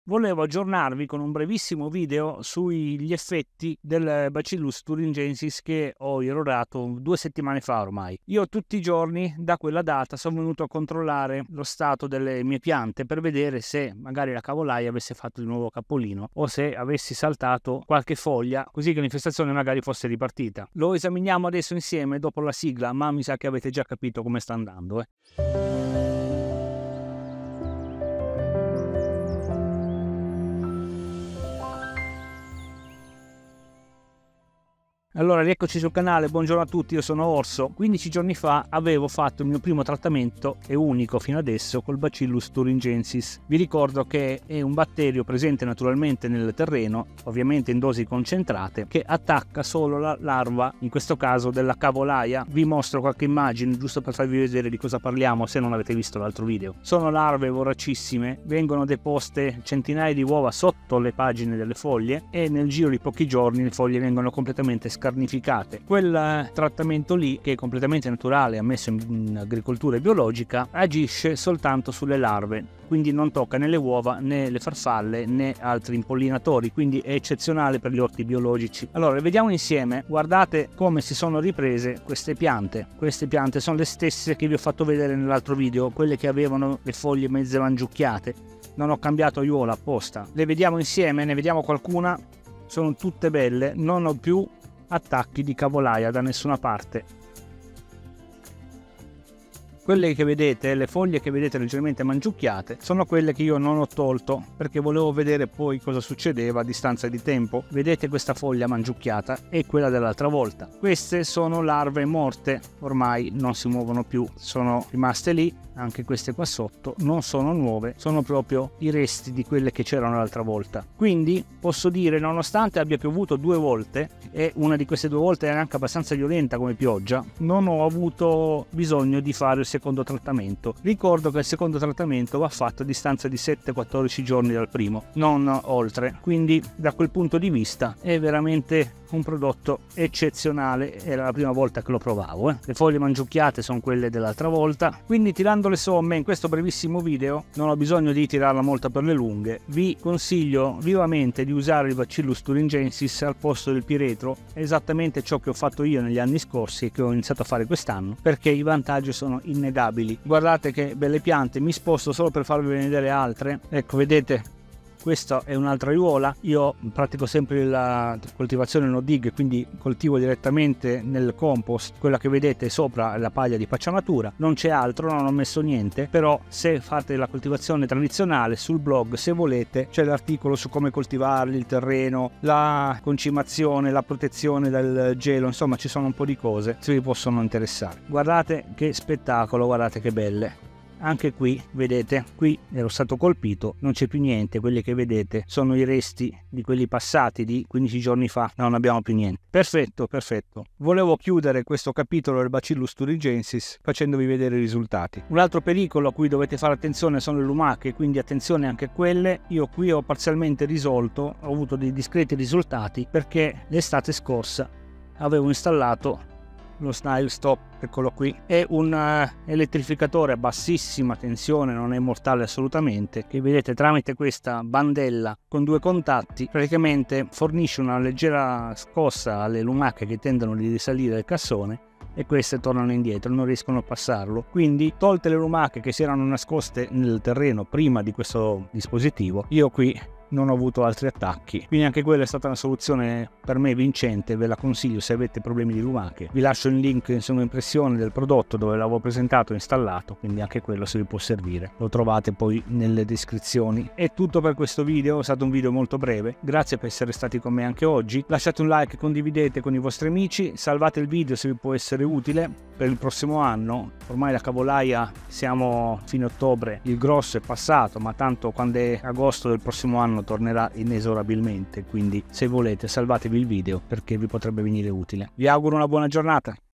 Bacillus Thuringiensis per la CAVOLAIA! Risultato dopo un solo trattamento!
In questo video vediamo i risultati dopo un solo trattamento di Bacillus Thuringiensis contro la cavolaia (bruchi dei cavoli) delle piante di cavolo.

Dopo un solo trattamento, possiamo vedere un miglioramento significativo della situazione. Le larve di cavolaia sono state uccise e le piante sono in salute.

*Dove acquistare lo Snail Stop
*Dove acquistare il Bacillus thuringiensis